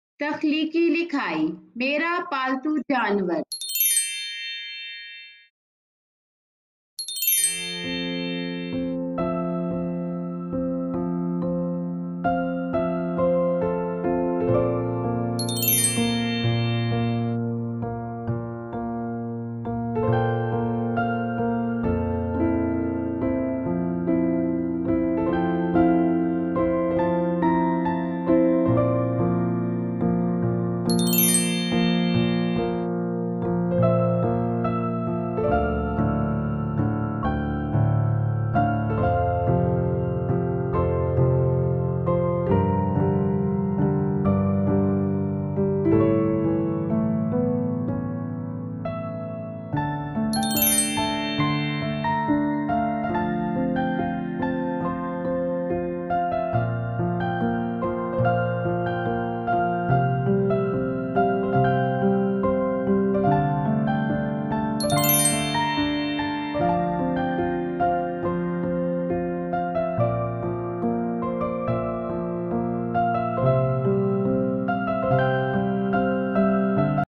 10 Lines on My Pet Animal in urdu | Mera Paltoo Janwar | Creative writing in urdu
10 Lines on My Pet Animal in urdu | Mera Paltoo Janwar | Creative writing in urdu | My Pet in urdu

Dear Parents and Teachers,